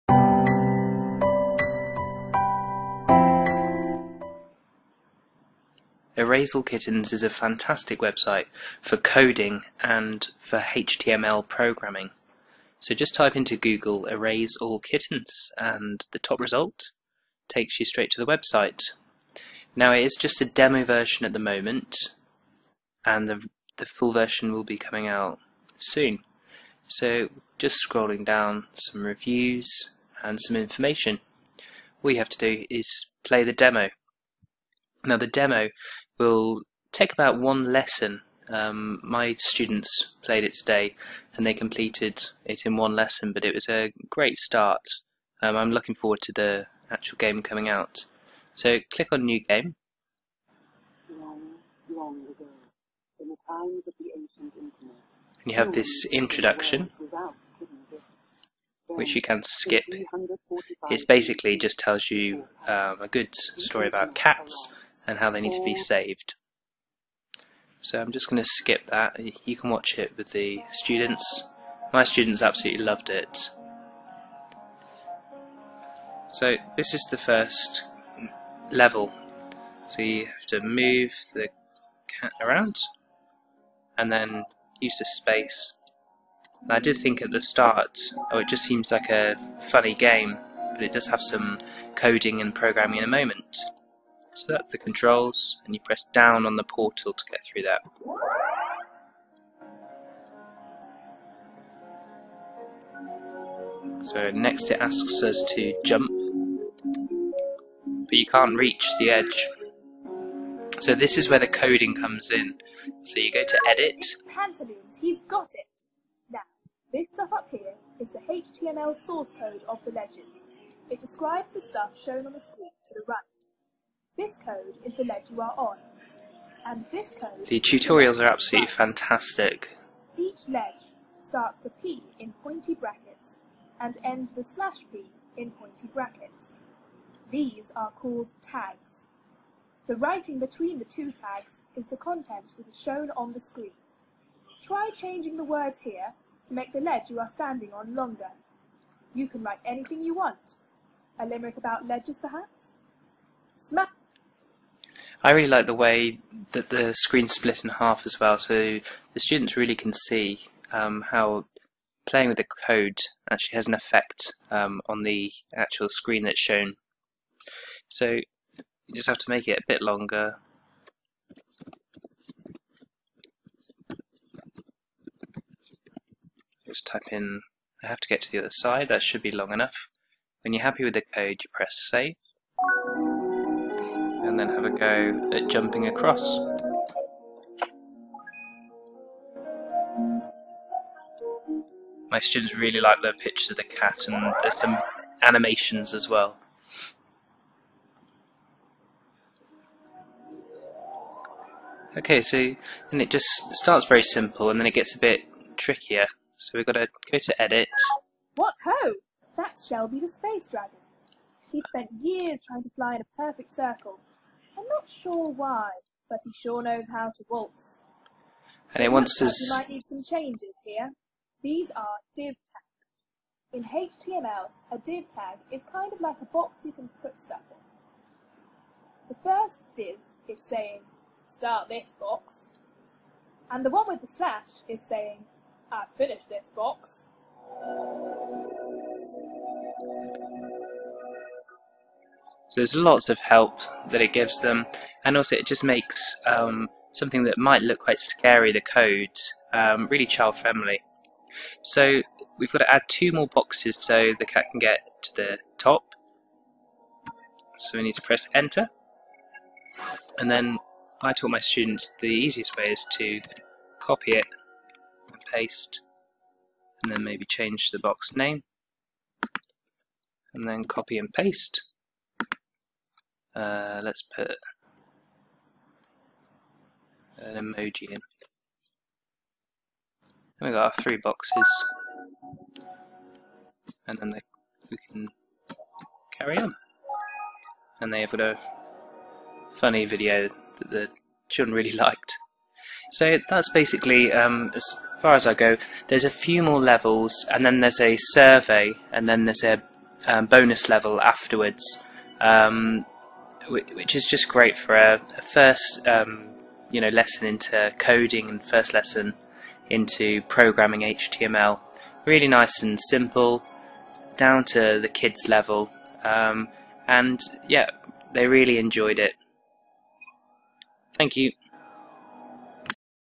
How To Use 'Erase All Kittens' To Teach Coding and HTML
Erase All Kittens is an absolutely fantastic website in which code and HTML are taught through gaming. My students absolutely LOVED it.

Here is a video tutorial on how to get started with the website.

Edtech4beginners.com